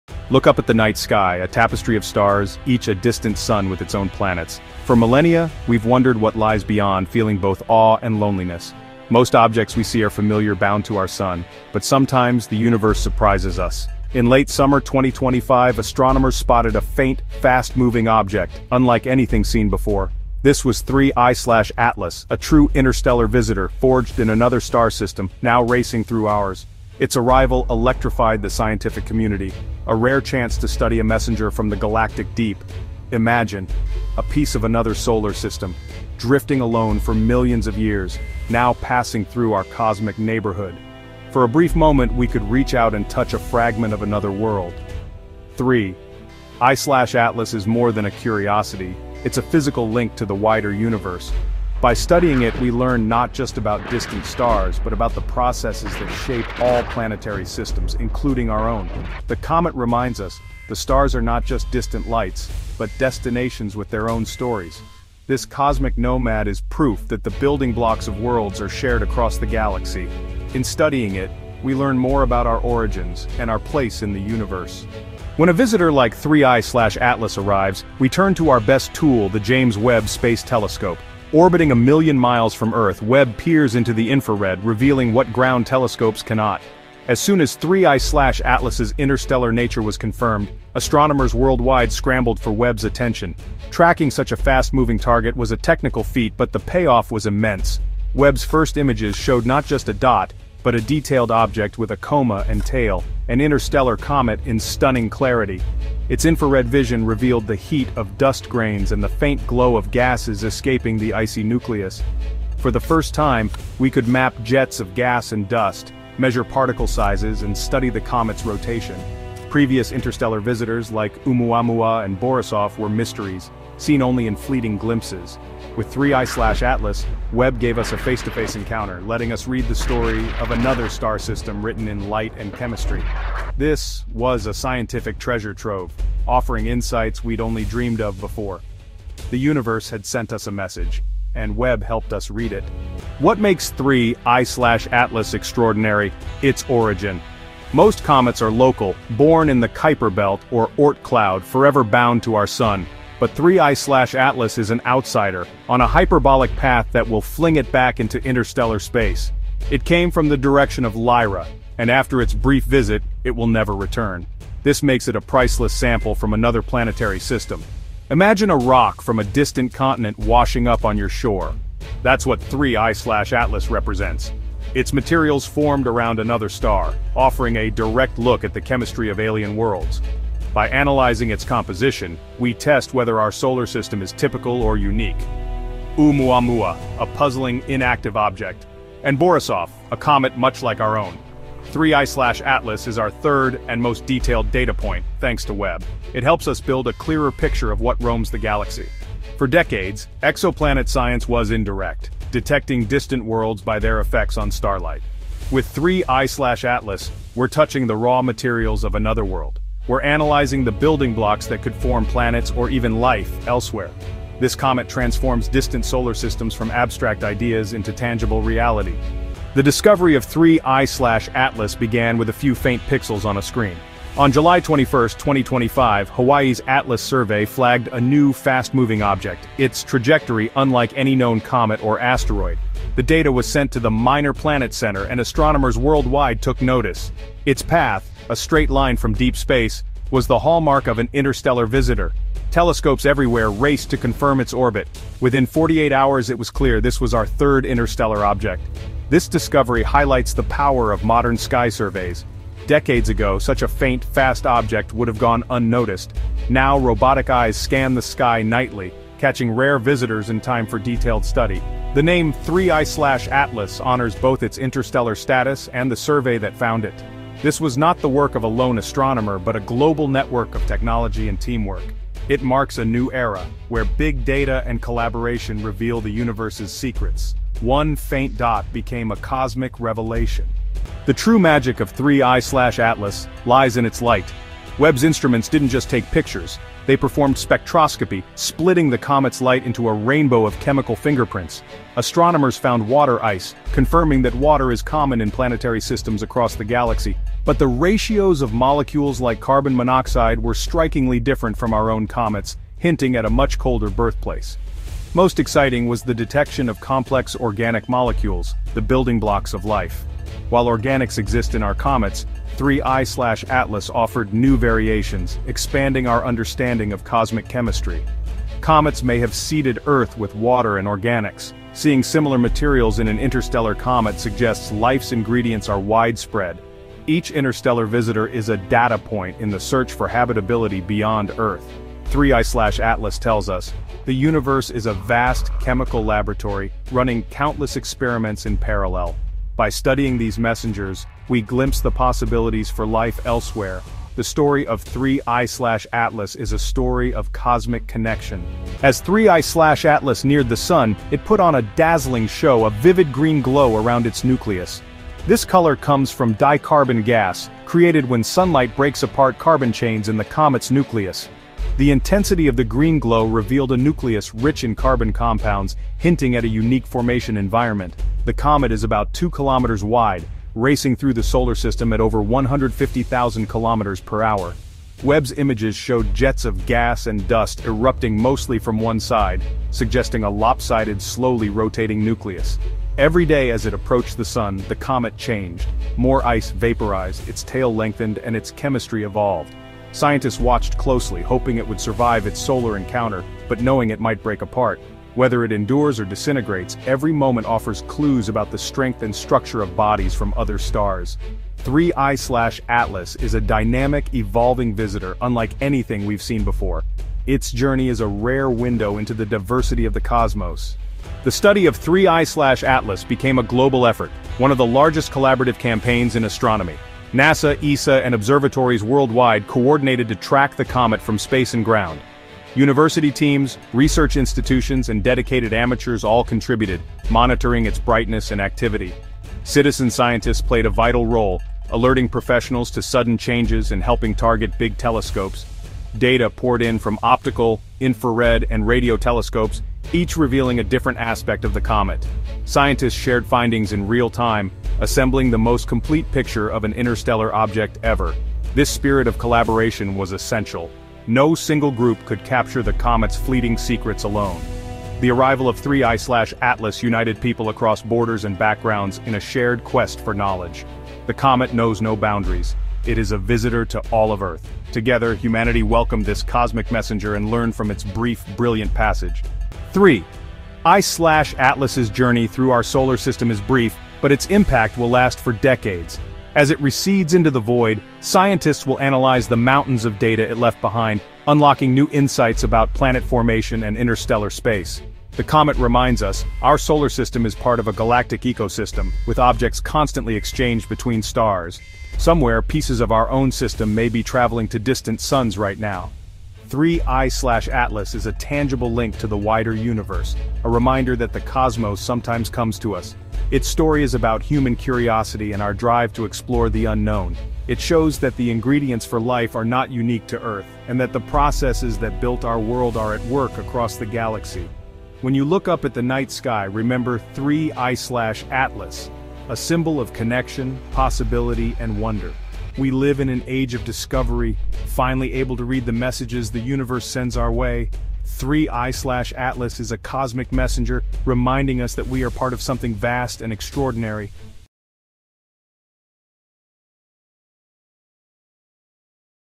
James Webb Releases the CLEAREST Image Yet of Interstellar Object 3I/ATLAS — Shocking View Uncovered
A comet from another star system, 3I/Atlas, has entered our solar system, giving scientists a rare chance to study an interstellar traveler.
The James Webb Space Telescope captured its most detailed images, revealing water, carbon dioxide, and unique chemical fingerprints.
Its massive seven-mile-wide core and mysterious green glow make it unlike any comet we’ve seen before.
Global telescopes, probes, and even amateur astronomers are uniting to track its journey across the inner solar system.
3I/Atlas is more than a comet—it’s a time capsule carrying secrets of another world and a reminder of our place in the galaxy.